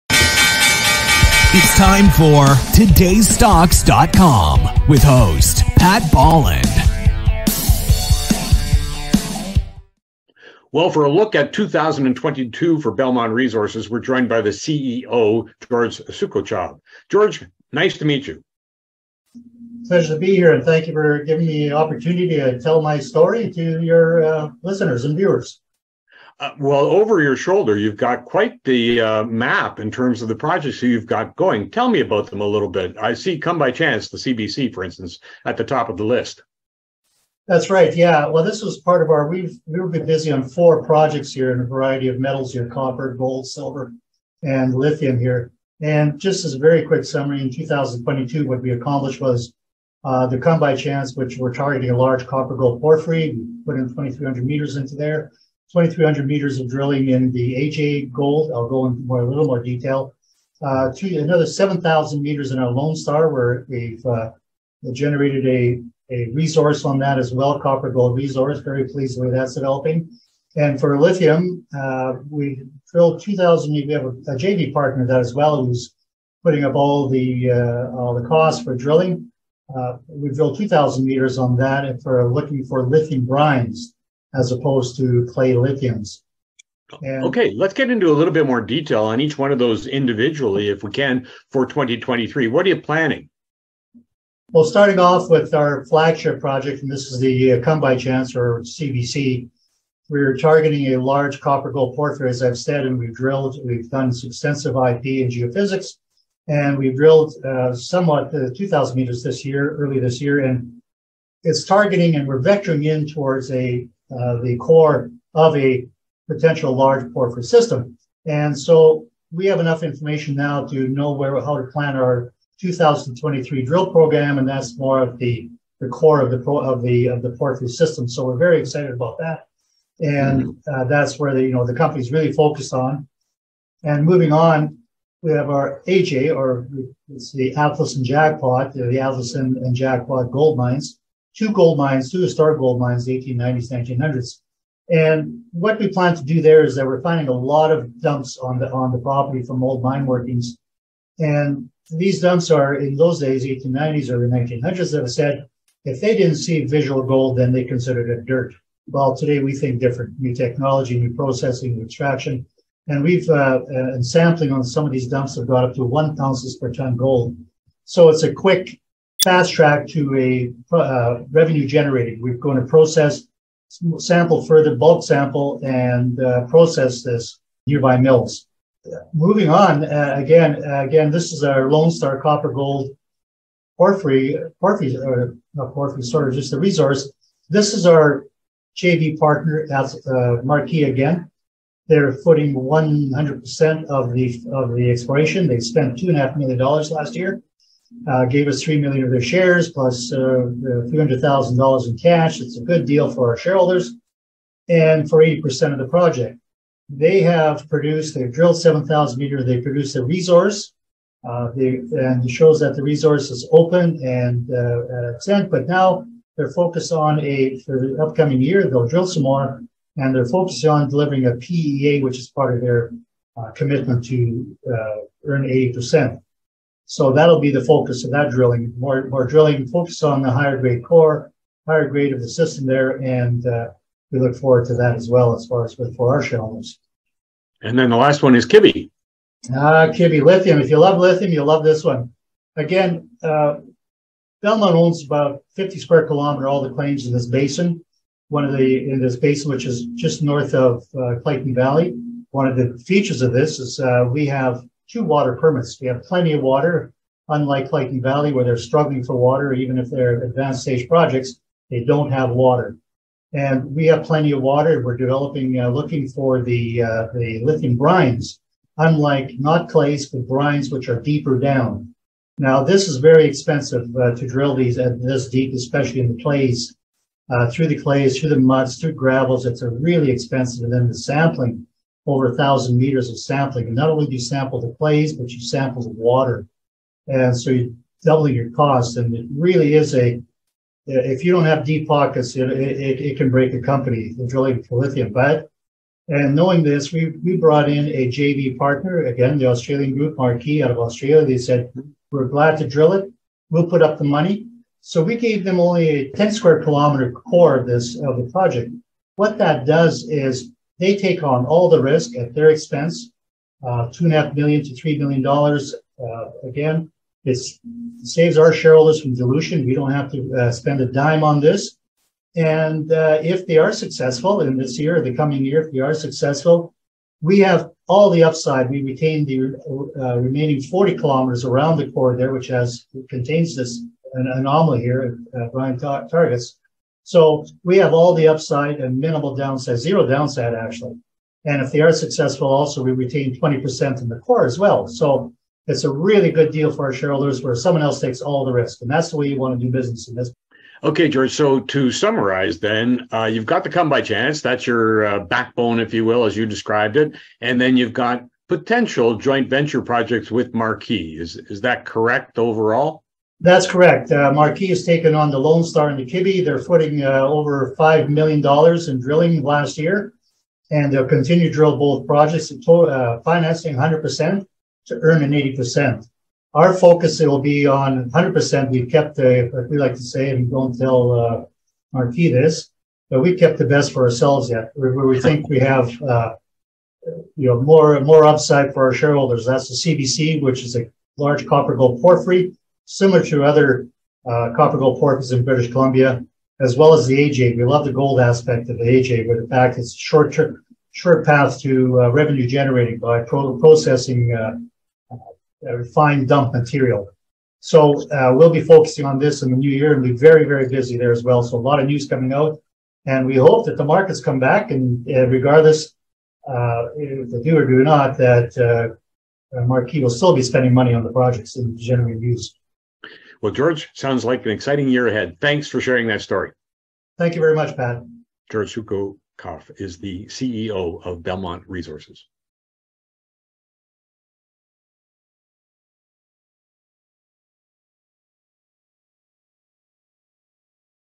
Belmont Resources is pleased to review the 2022 year of exploration on its projects.
Belmont CEO George Sookochoff was interviewed by Mr. Pat Bollard of TodaysStocks.com where he discusses the achievements of 2022